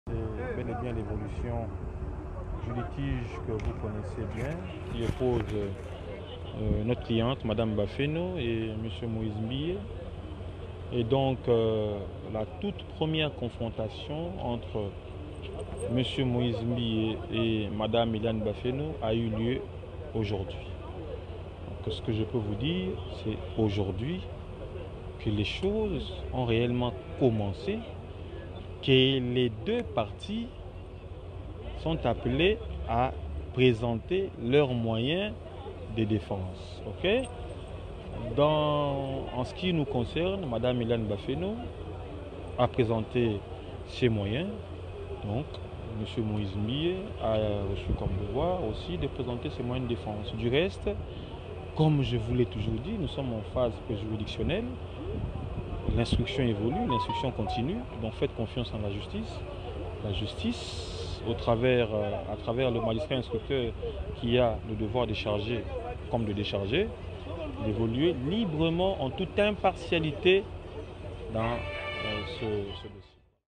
AVOCAT ELIANE ALOBI PONA VIDEO YA MUTAKALA NA YE NA ELIANE
DOSSIER ELIANE ET PASTEUR  MOISE MBIYE